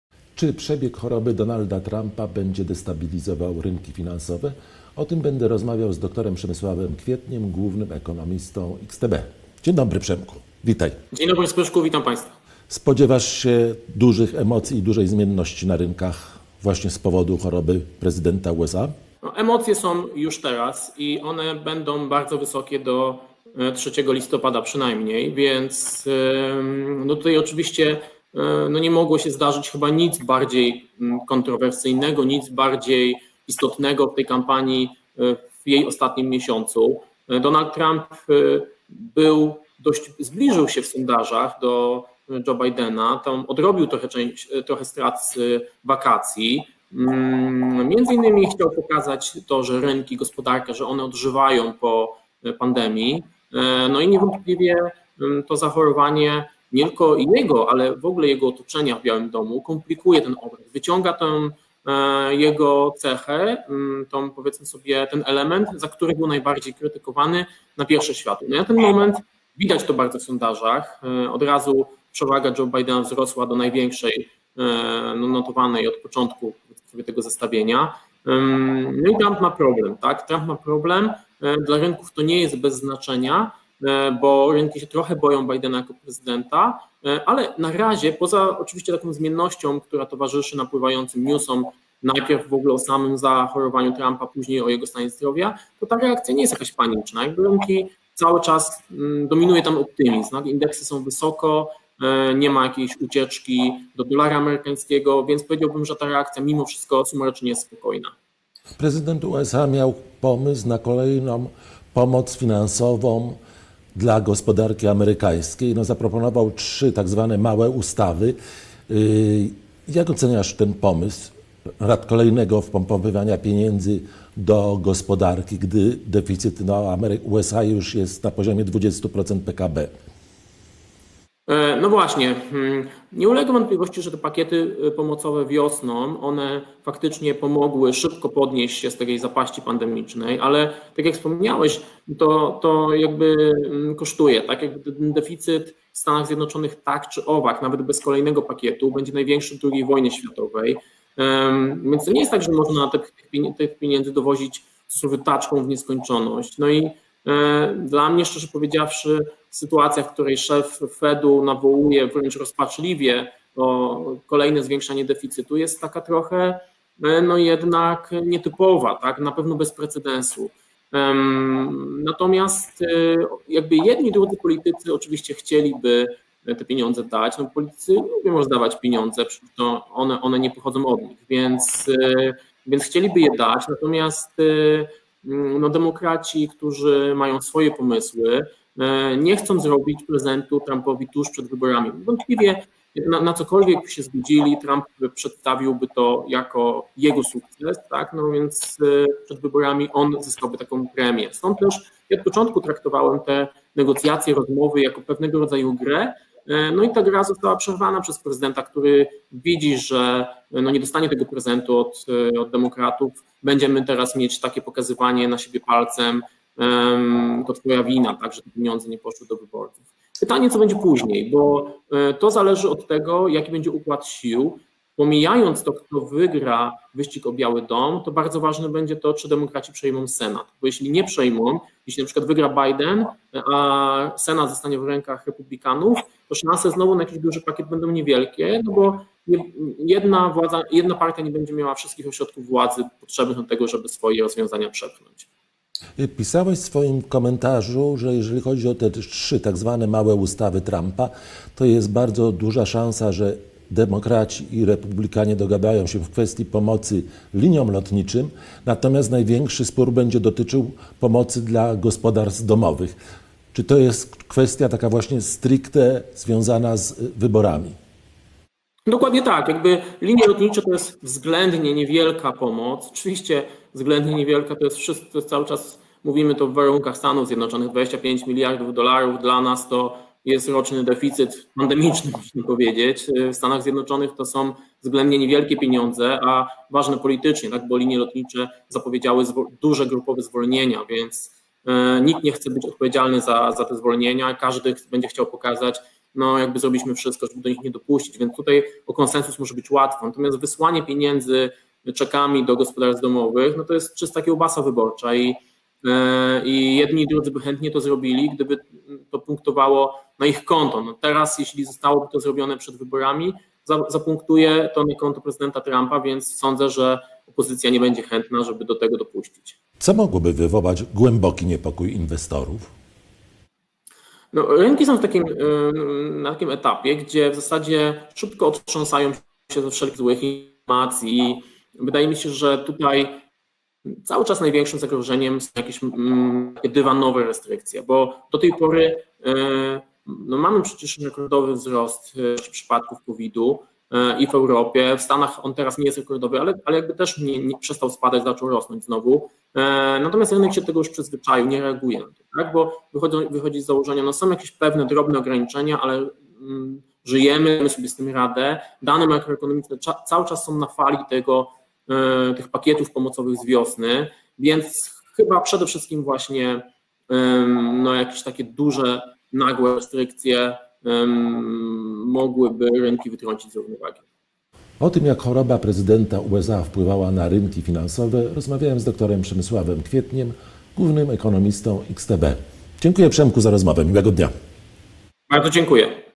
Jak wygląda sytuacja na trzy tygodnie przed wyborami prezydenckimi w USA
Przebieg choroby Donalda Trumpa zaskakująco słabo wpływa na zmienność na rynkach finansowych. Wiele emocji w USA wywołuje natomiast spór dotyczący dodatkowej pomocy finansowej dla gospodarstw domowych.

- Donald Trump chciał pokazać w kampanii, że rynki finansowe odżywają po pandemii, a więc jego choroba ten plan bardzo komplikuje – mówi w rozmowie z MarketNews24 dr Przemysław Kwiecień, główny ekonomista XTB. – Newsy o przebiegu choroby prezydenta wywołują wahania na giełdach, ale reakcje rynków finansowych nie są panicze.

Indeksy giełdowe są nadal na wysokich poziomach i nie ma ucieczki od akcji do dolara amerykańskiego.

W kontekście wyborów warto zwrócić uwagę na fakt, iż szanse Donalda Trumpa w ostatnim czasie zmalały, choć dokładnie cztery lata temu - w tym samym przedwyborczym czasie - Clinton osiągnęła swoje największe poparcie. Taka sama sytuacja może się powtórzyć w przypadku Bidena.

Co ciekawe, dalszy impas dotyczący kolejnej pomocy dla gospodarki od amerykańskich władz nie powoduje złych nastrojów. Zdaniem uczestników rynku można oczekiwać, że nowy rok i nowa administracja doprowadzą sprawę do końca. Oczywiście problem może mieć miejsce w takim przypadku jak np. wygrana Bidena oraz brak większości dla Demokratów w Kongresie. Niemniej rynek w tym momencie raczej na to nie patrzy.

Pomiędzy Demokratami i Republikanami trwa obecnie spór o zakres dalszej pomocy finansowej wpompowywanej w gospodarkę. Obecny prezydent zaproponował tzw. trzy małe ustawy, a najbardziej konfliktowa jest ta, która dotyczy pomocy finansowej dla gospodarstw domowych.

- Wysłanie czeków finansowych do gospodarstw domowych jest to typowa „kiełbasa wyborcza”, jedni i drudzy chętnie by to zrobili, gdyby tali sukces mogli przypisać sobie na wyłączność – komentuje ekspert XTB.